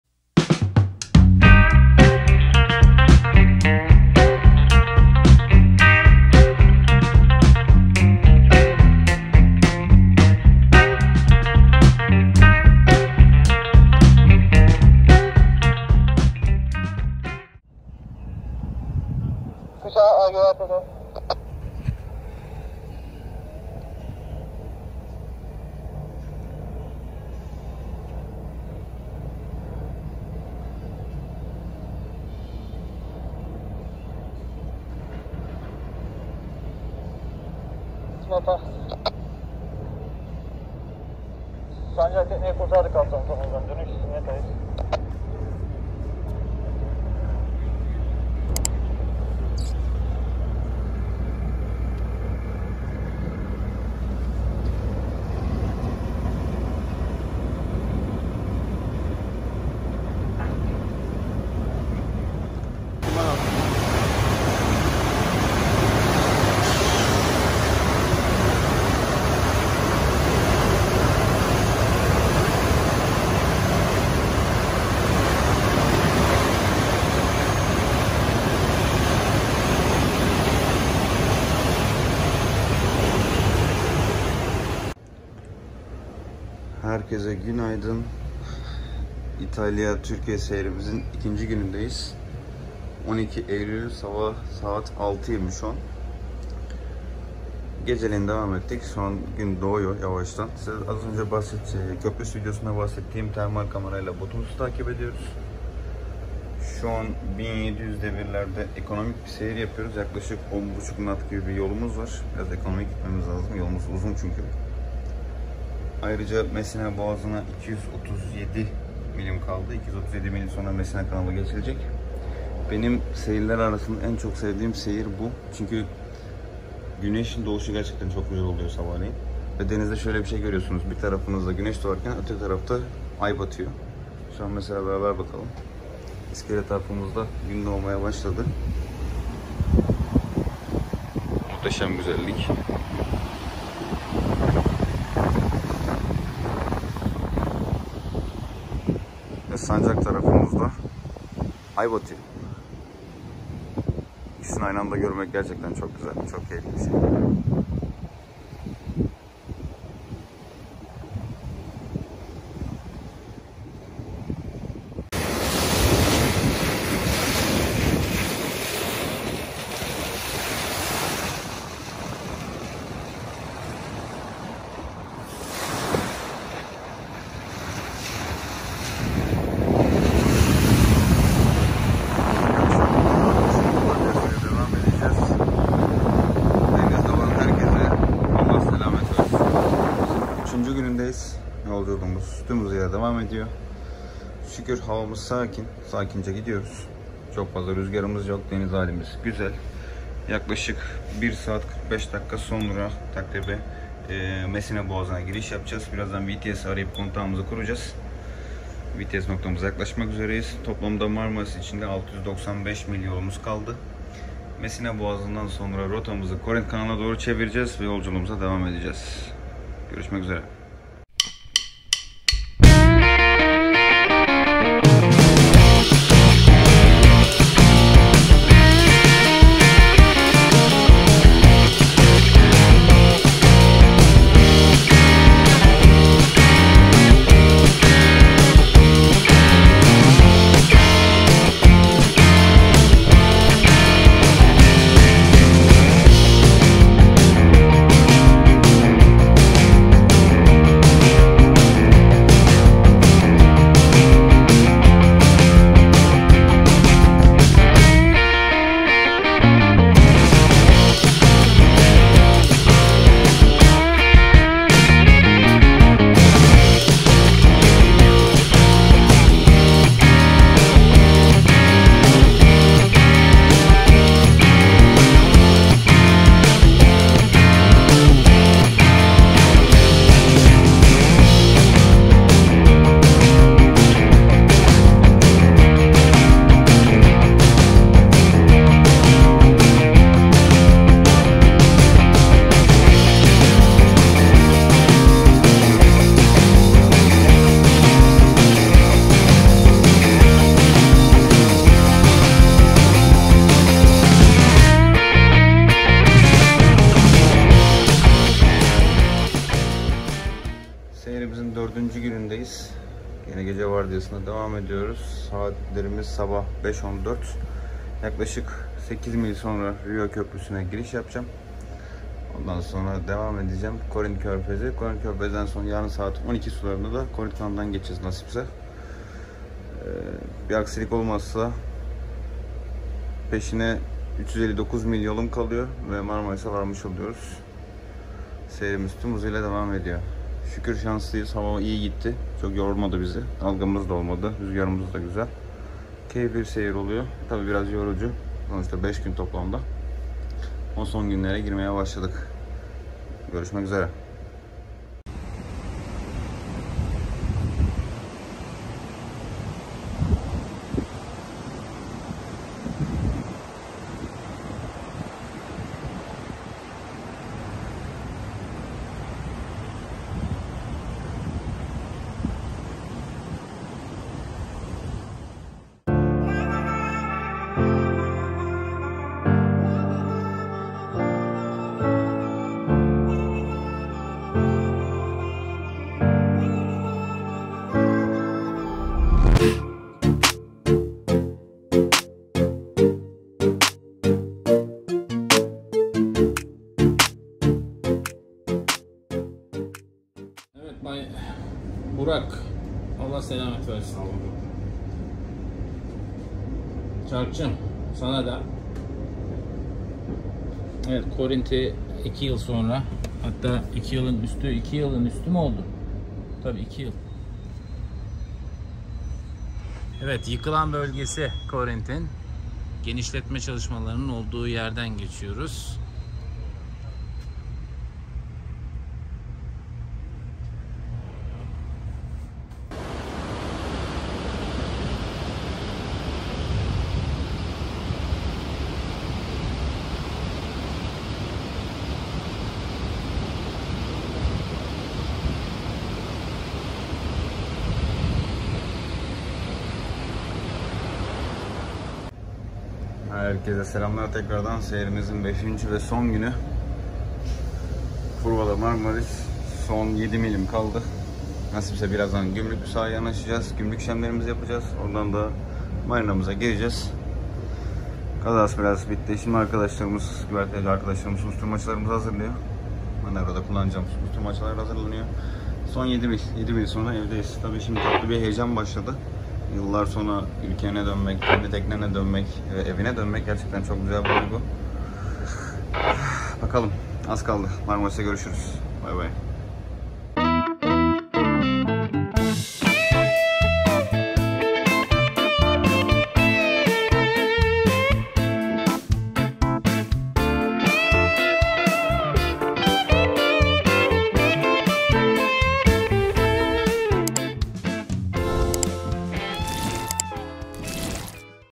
İtalyadan türkiyeye tekne yolculuğu vlog - Yat kullanmak uzun seyirler
Herkese merhabalar bu videomuzda sizlerle İtalya Türkiye arasındaki 5 günlük yolculuğumuz ve seyrimiz den bahsetmek istedim uzun yıllar sonra evde olmak gerçekten çok güzel